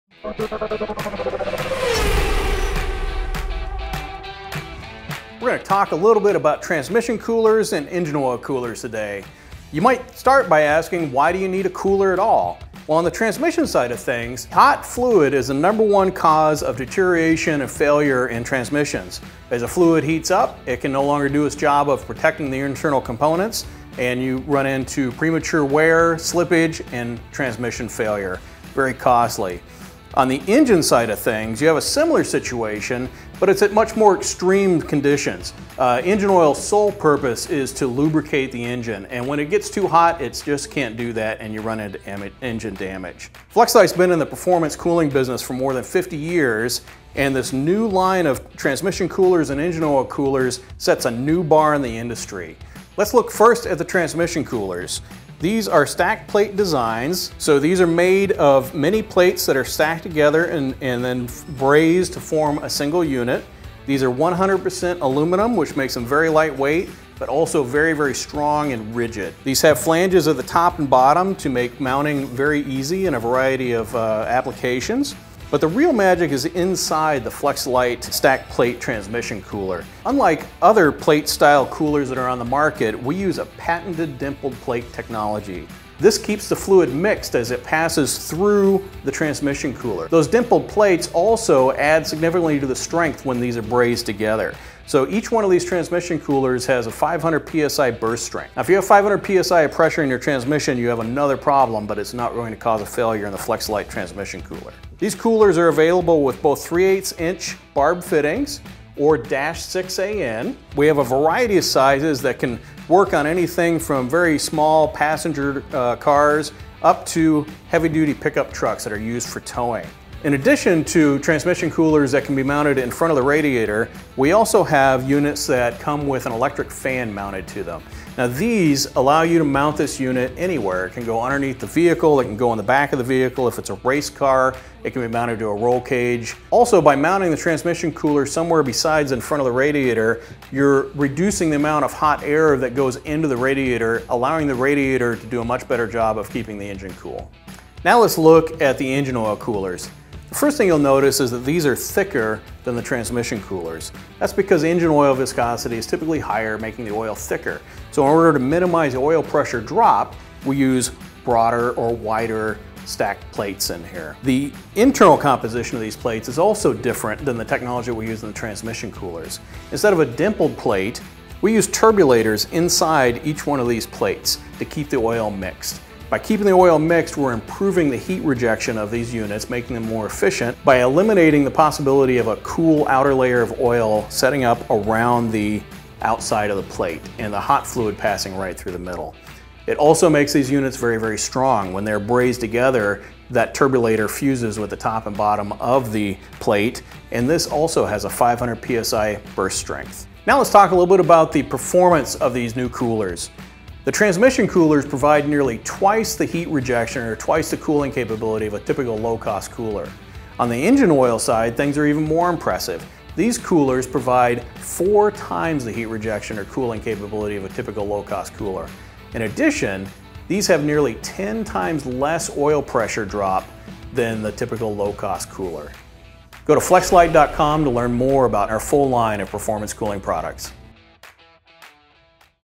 Flex-A-Lite Transmission and Engine-Oil Coolers Overview
Flex-A-Lite has launched a new line of advanced stacked-plate transmission and engine-oil coolers that offer better cooling and stronger constructions. This video explains how the patented dimpled plate technology in the new Flex-A-Lite transmission coolers provide nearly twice the heat rejection (cooling) and the turbulizors in the engine-oil coolers give 10 times better heat rejection than low-cost coolers.